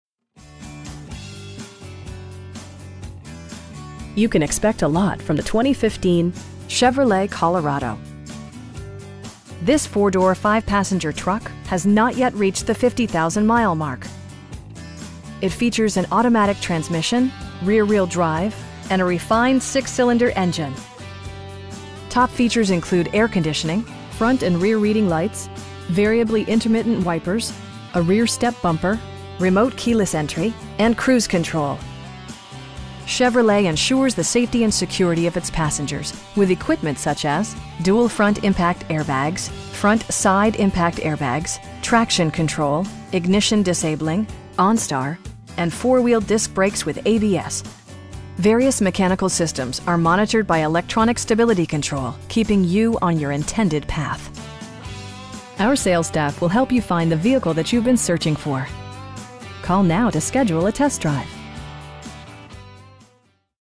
2015 Chevrolet Colorado 2WD LT in Tallahassee, FL 32308
Dale Earnhardt Jr. Buick GMC
1850 Capital Circle NE

You can expect a lot from the 2015 Chevrolet Colorado. This 4 door, 5 passenger truck has not yet reached the 50,000 mile mark! It features an automatic transmission, rear-wheel drive, and a refined 6 cylinder engine. Top features include air conditioning, front and rear reading lights, variably intermittent wipers, a rear step bumper, front bucket seats, telescoping steering wheel, remote keyless entry, and cruise control. Chevrolet ensures the safety and security of its passengers with equipment such as: dual front impact airbags, front side impact airbags, traction control, ignition disabling, onStar, and 4 wheel disc brakes with ABS. Various mechanical systems are monitored by electronic stability control, keeping you on your intended path. Our sales staff will help you find the vehicle that you've been searching for. We'd be happy to answer any questions that you may have.